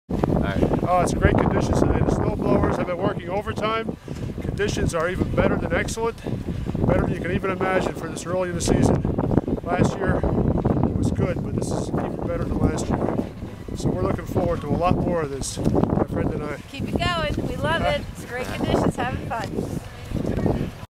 4pm Update 12.15.10
Conditions at Vernon continue to be great. We are continuing to blowing snow through the week.